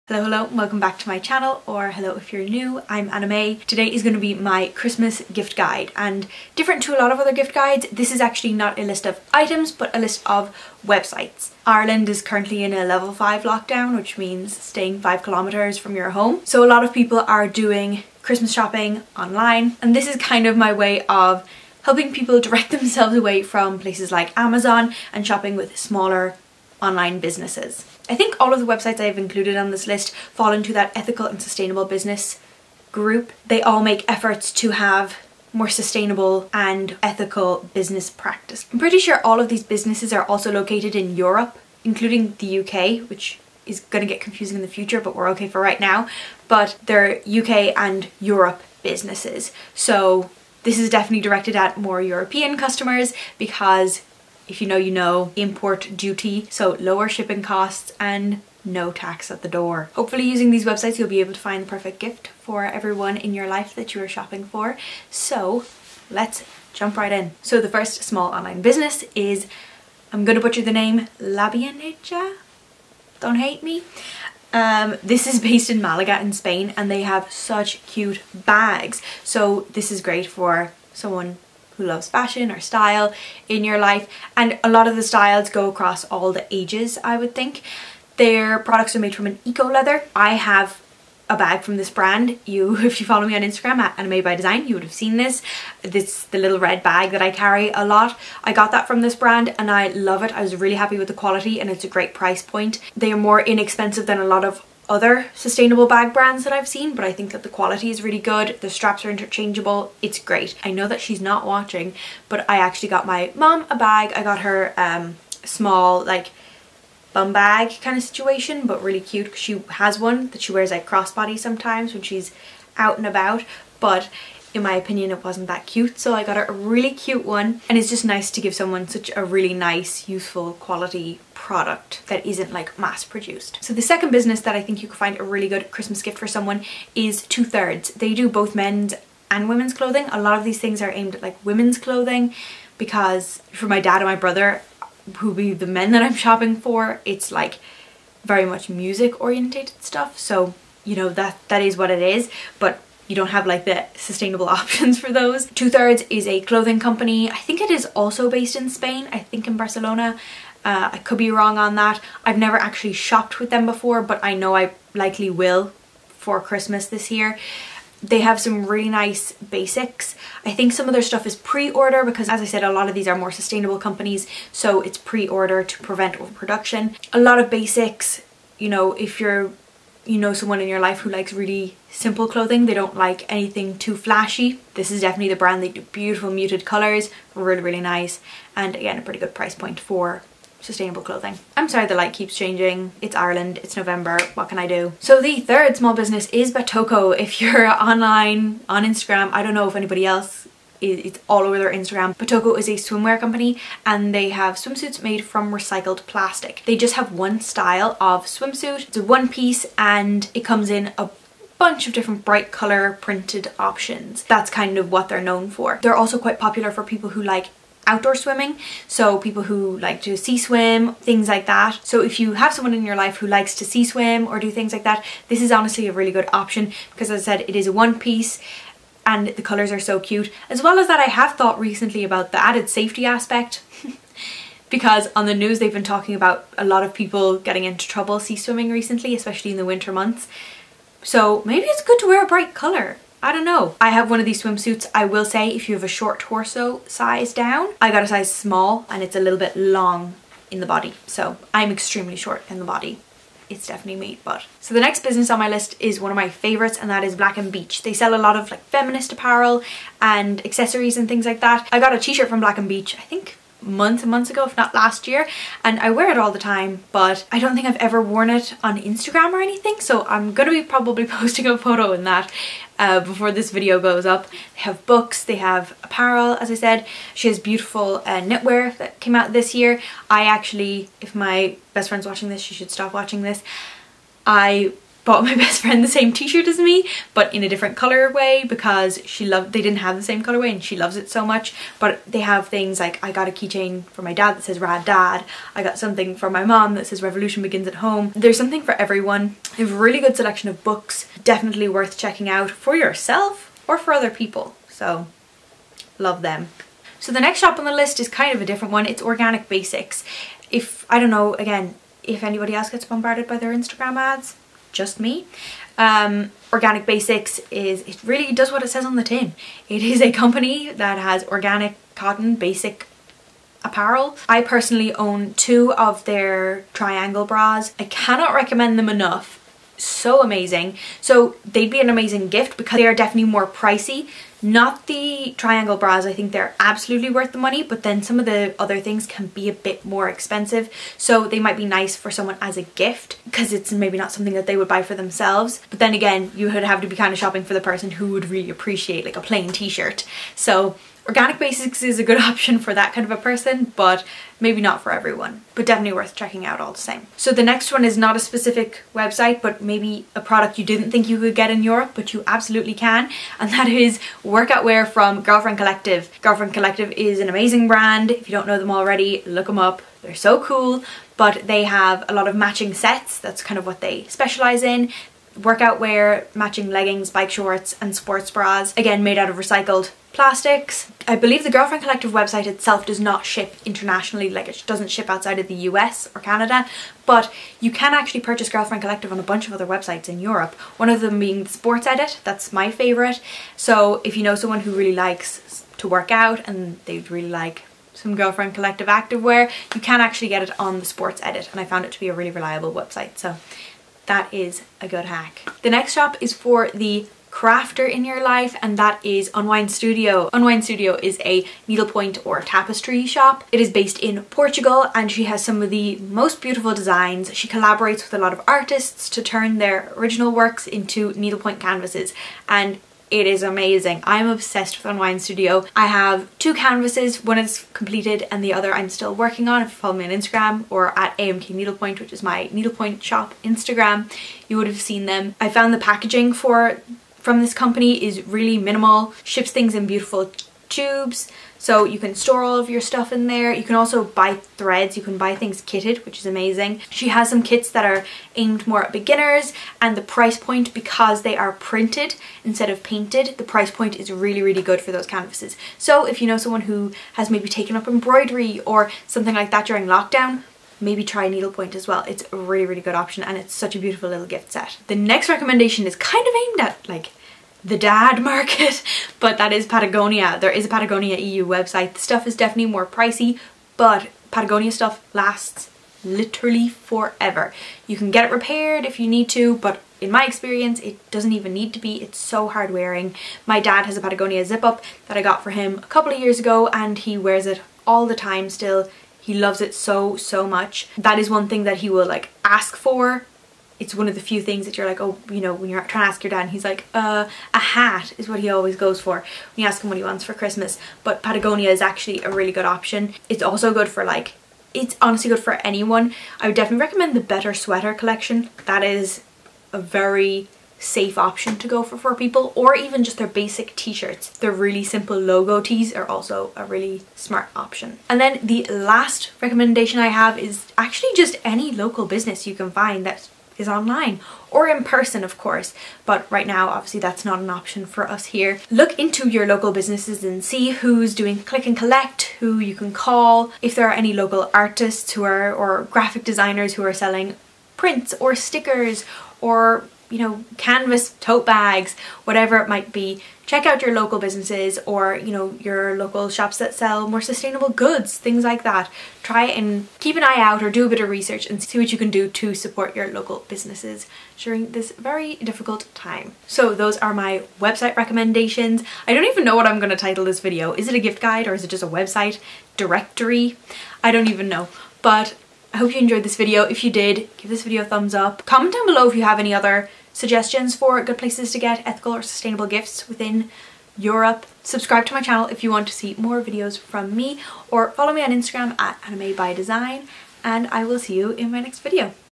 GIFT GUIDE | ethical/sustainable online shops in Europe
A lot of people are doing their best to shop more intentionally this year, choosing small businesses and/or businesses with more sustainable or ethical practices.  I wanted to share a list of my favourite online shops (in Europe) that are perfect for finding Christmas gifts!

My Etsy Shop / @amk_needlepoint